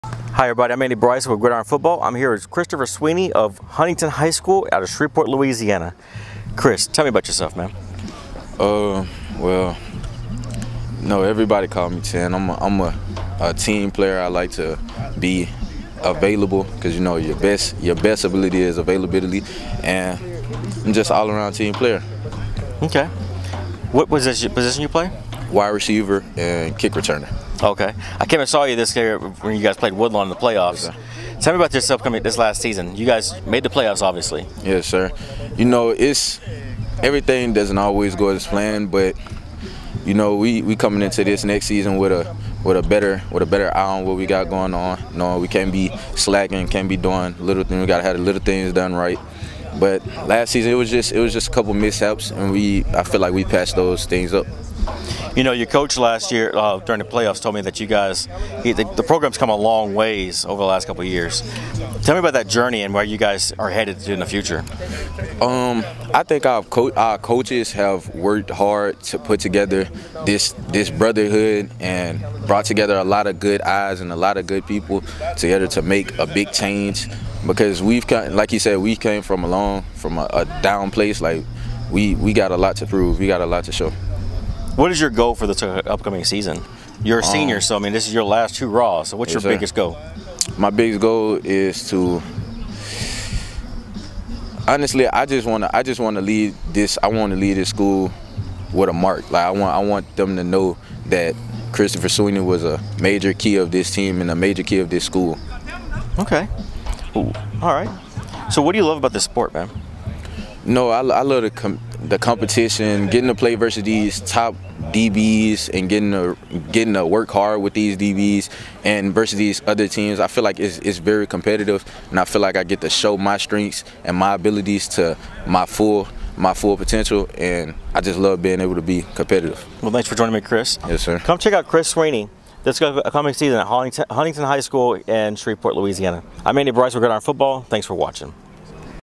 Huntington RB Christopher Swinney
Gridiron Football CEO Andy Bryson interviews Huntington High School 2025 RB Christopher Swinney at the Gridiron Football Elite Camp held at Bossier High School! More from this camp will be posted on our website and our digital magazine! to keep updated with all of our content!